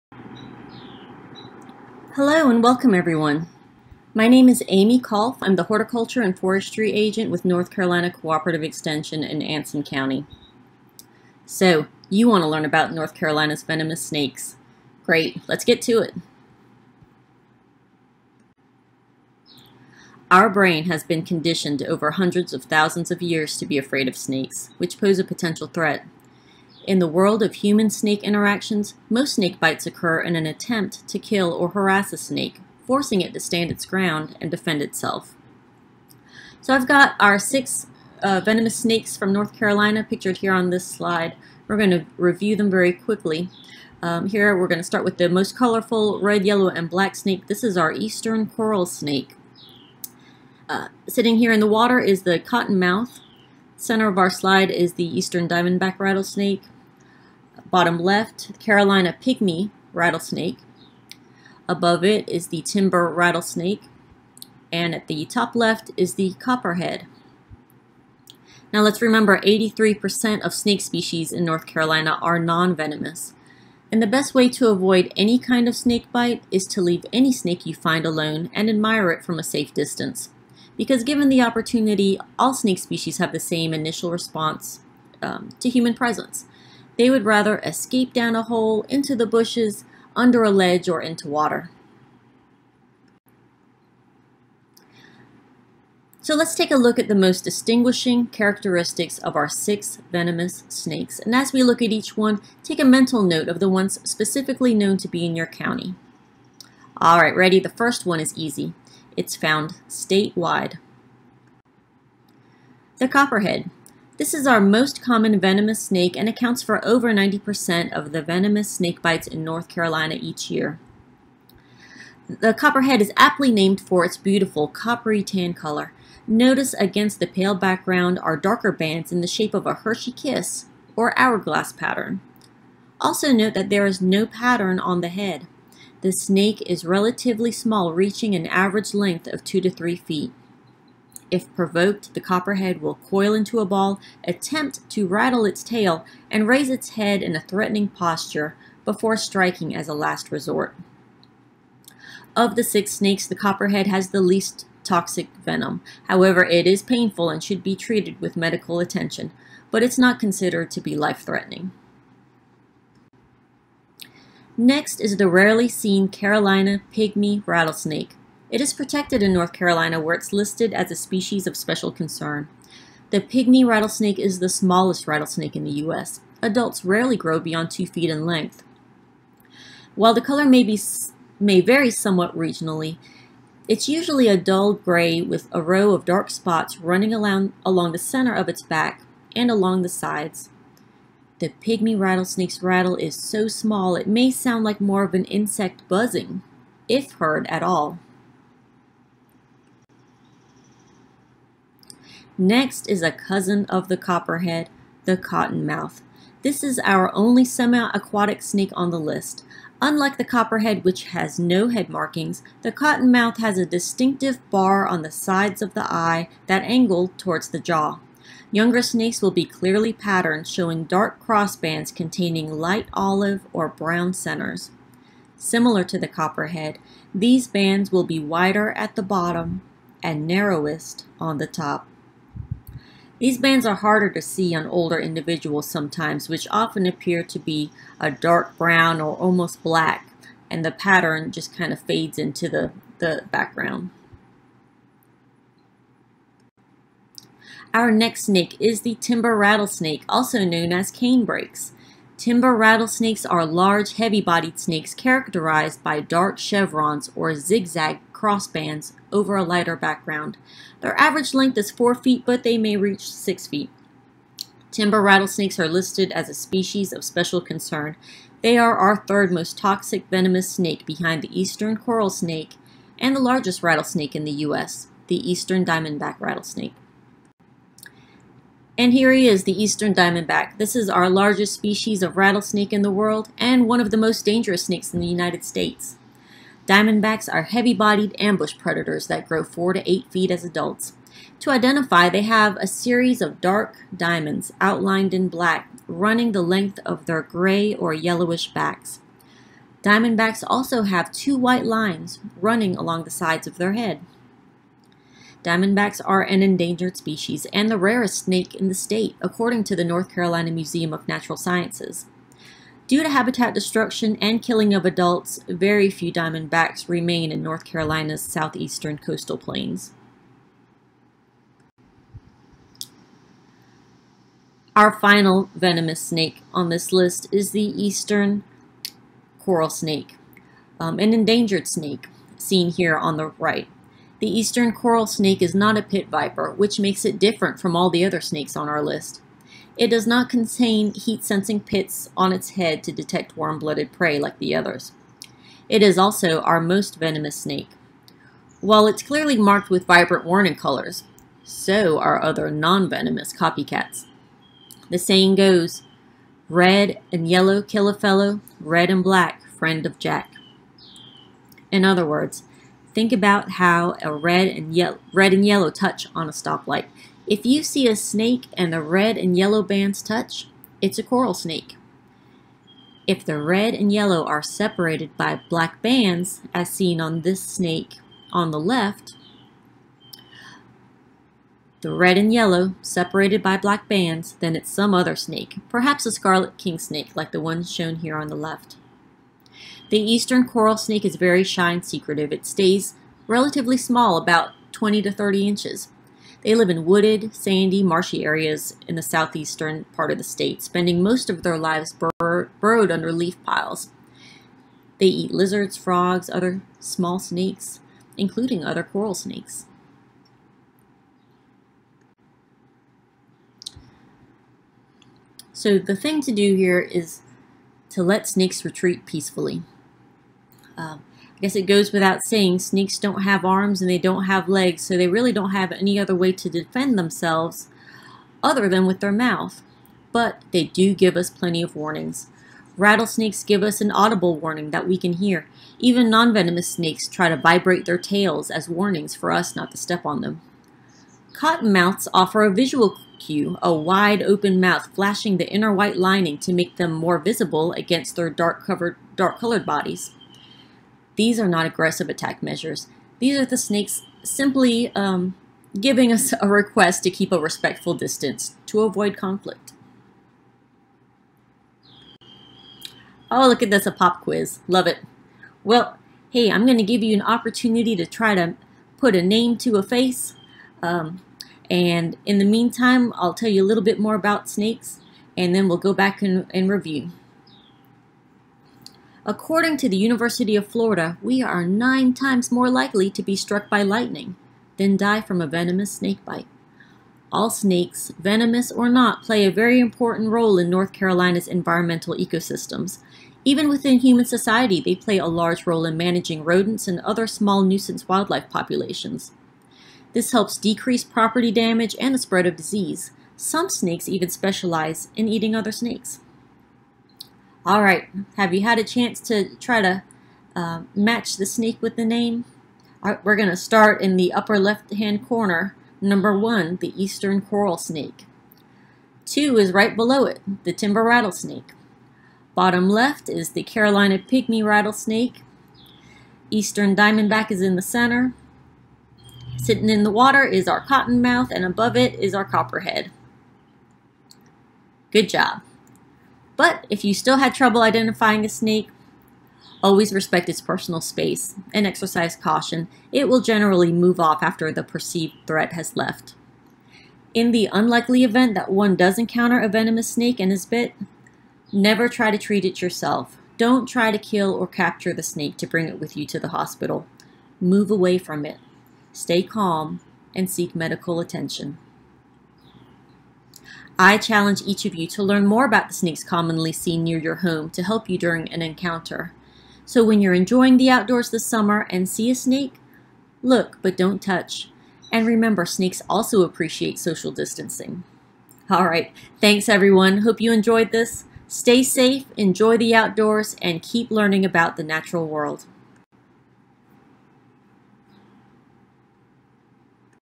North Carolina's Venomous Snakes
North Carolina's Venomous Snakes as presented by Anson County Cooperative Extension's Horticulture Aimee Colf.